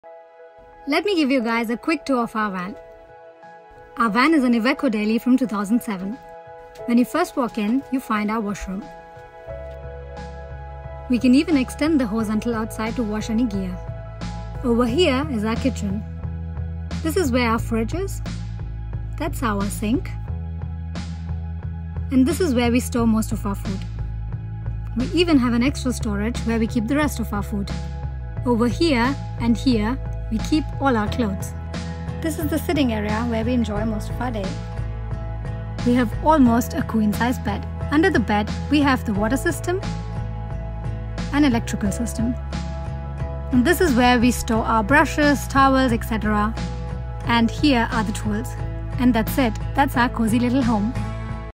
Tour of our tiny home | VAN TOUR (in 50 sec)
Over a period of 1 year, we converted this cargo van into our cosy little home.

We have built it completely ourselves and are very happy with how it came out.
Let us know what you think about it 🙂

For a full van tour, head over to our YouTube channel.

Now we are traveling with our home around Europe and loving the views on the road
Follow us for more vanlife adventures 🤩